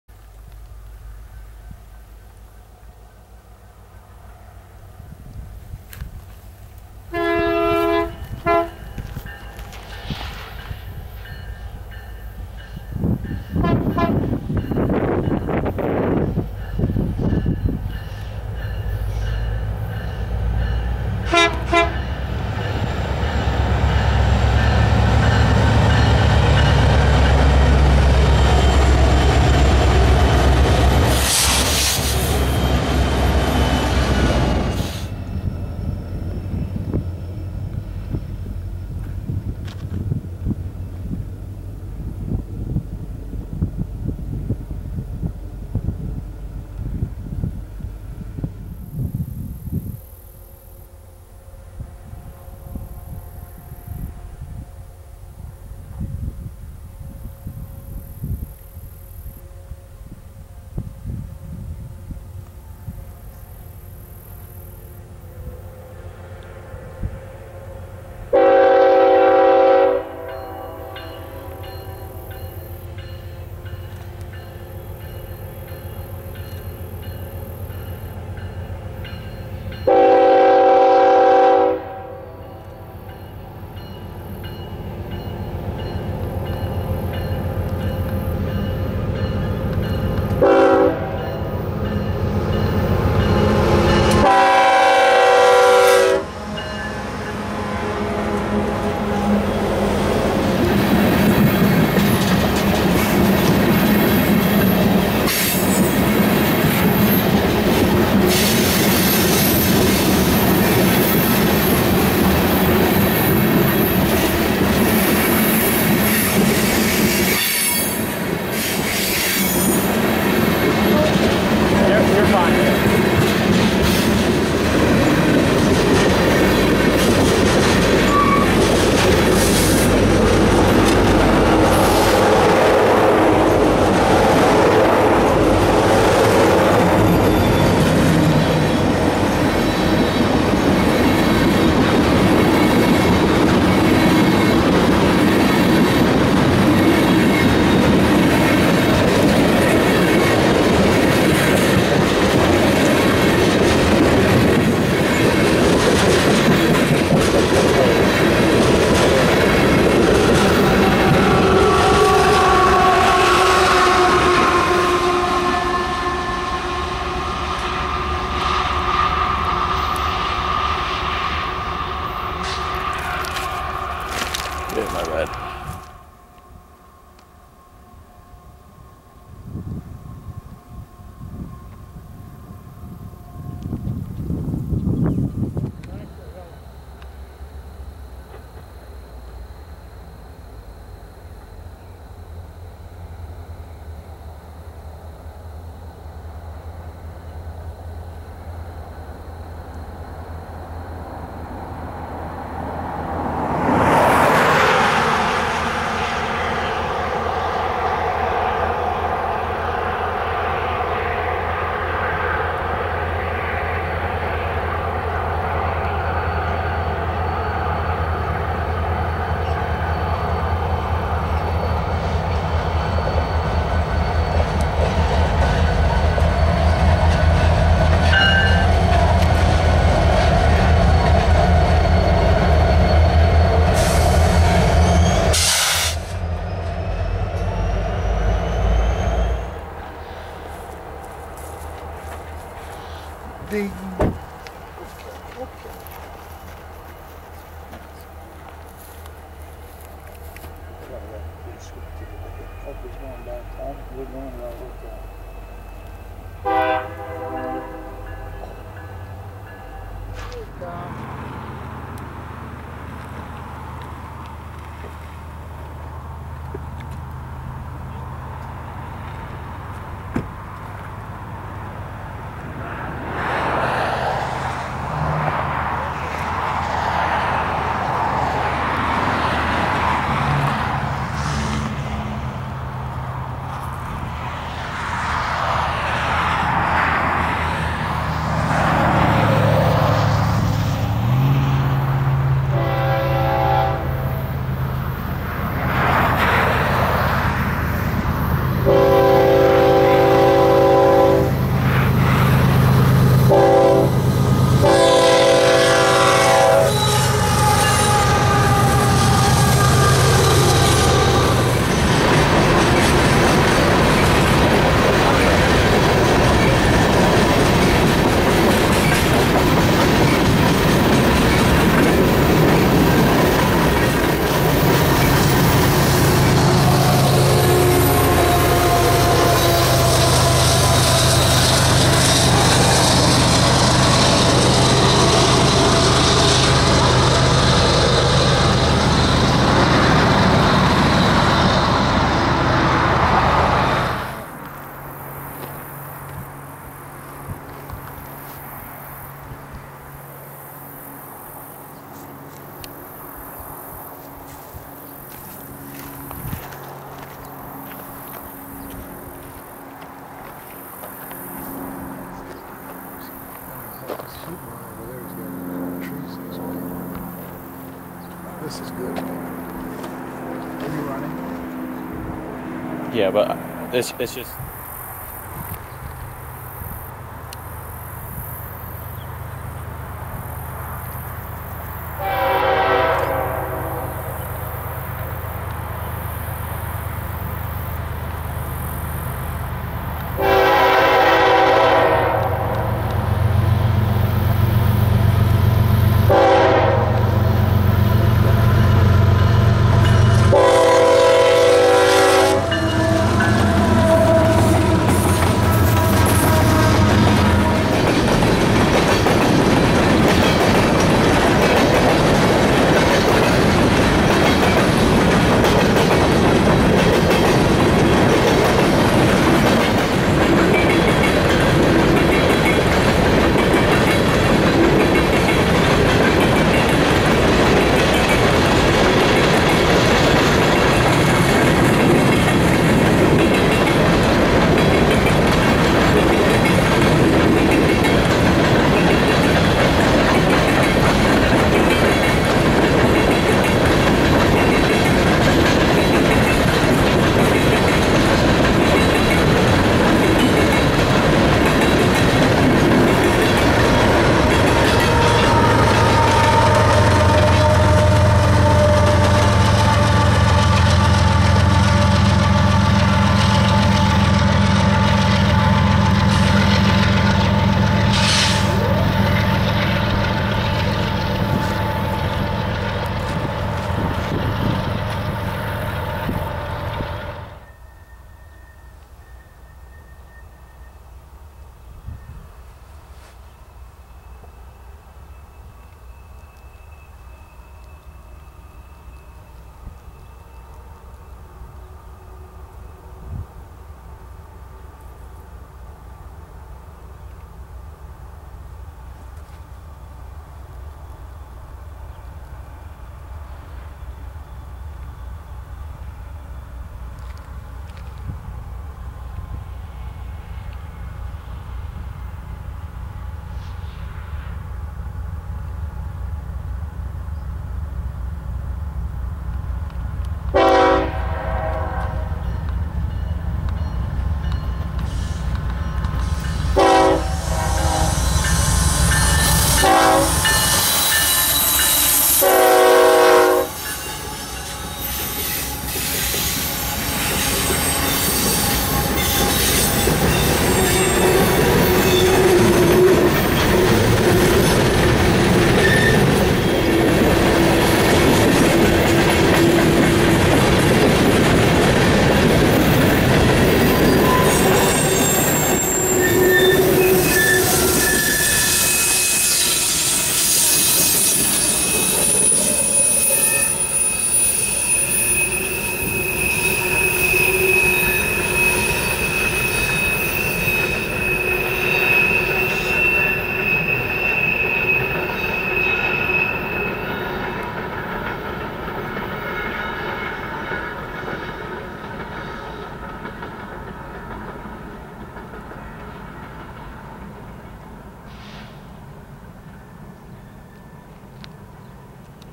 MRL's Mullan Pass (and more) - Citirail, Executive MACs, and Ferromex
Here's another installment of this summer's railfan adventures. After railfanning in Livingston, we went west to Helena and Mullan Pass. After waiting forever and catching the same that we did on Bozeman pass, we decided to continue west where we slept over in Missoula. The next day, we took a short detour to check out the Garden of 1000 Buddhas in Arlee, MT. As we were leaving, we caught a fast moving SB coal train headed back to Missoula, so we chased it back, catching it in Arlee, over Evaro Hill, and in some horseshoe curves closer to Missoula.
Hope you enjoy and look for more action from our trip in the next video!
-Cam